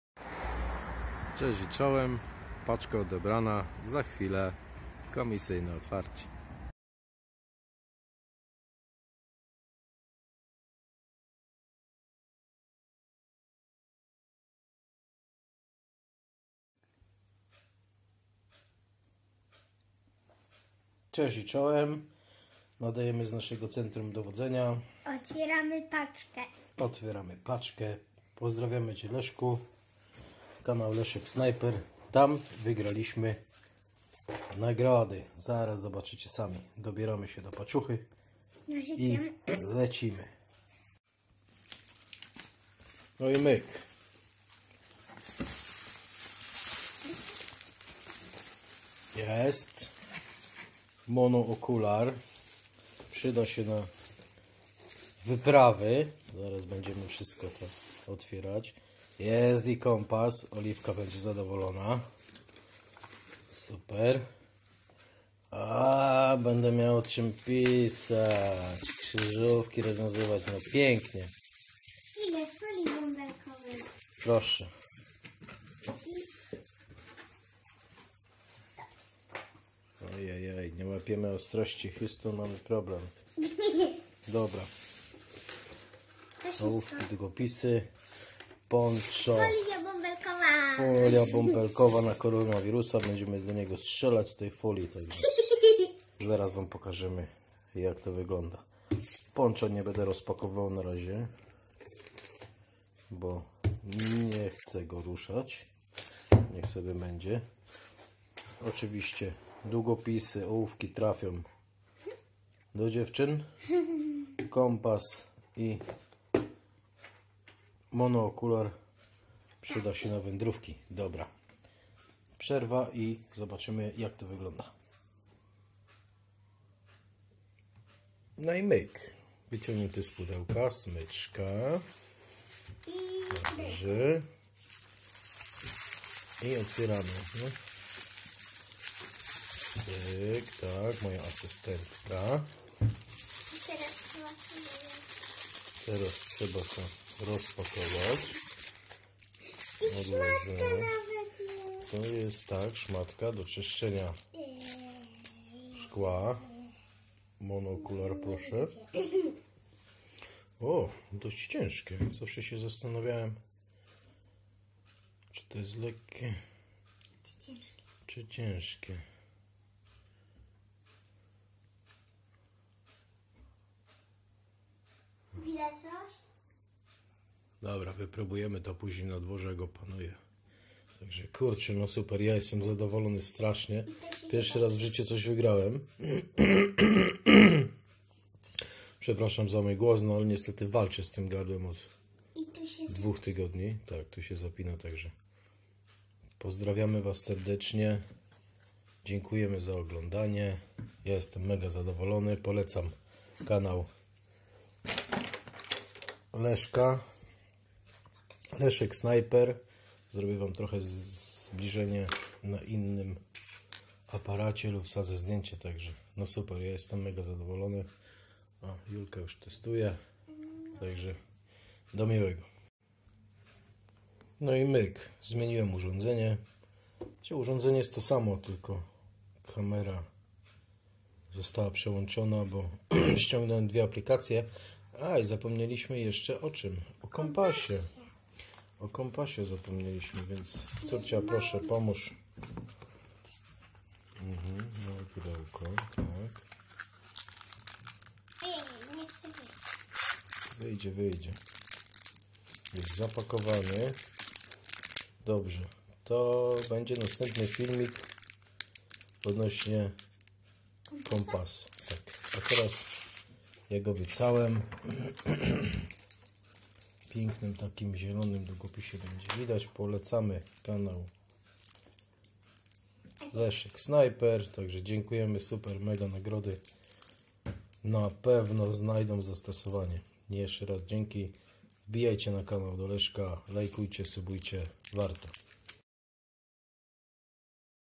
Nagroda dotarła polecam kanał Leszek Snajper
Pierwszy raz coś wygrałem 😲 i bardzo się cieszę z tych nagród niedługo film z testowania w parku VideoShow